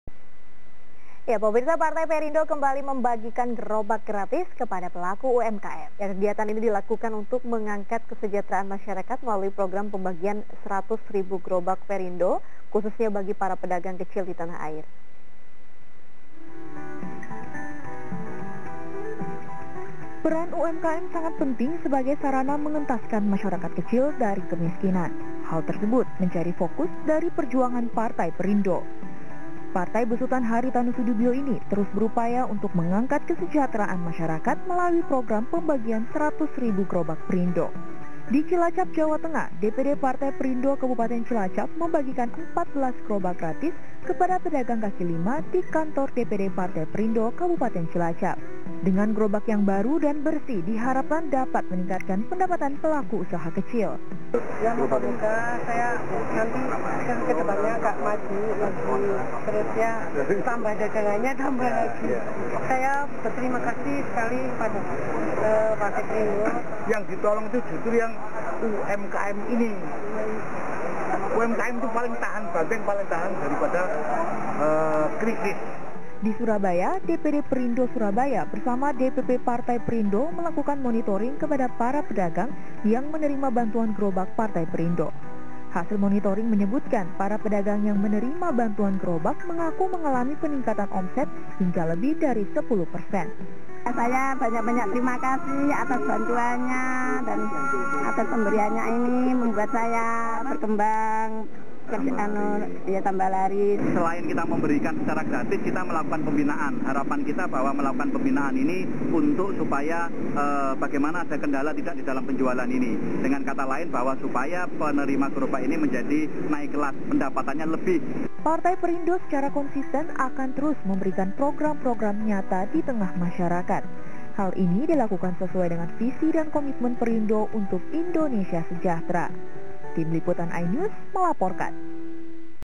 Perindo Gelar Program Pembagian 100 Ribu Gerobak
Partai Perindo kembali membagikan gerobak gratis kepada pelaku UMKM, kegiatan ini dilakukan untuk mengangkat kesejahteraan masyarakat melalui program pembagian 100 ribu gerobak Perindo khususnya bagi para pedagang kecil di tanah air.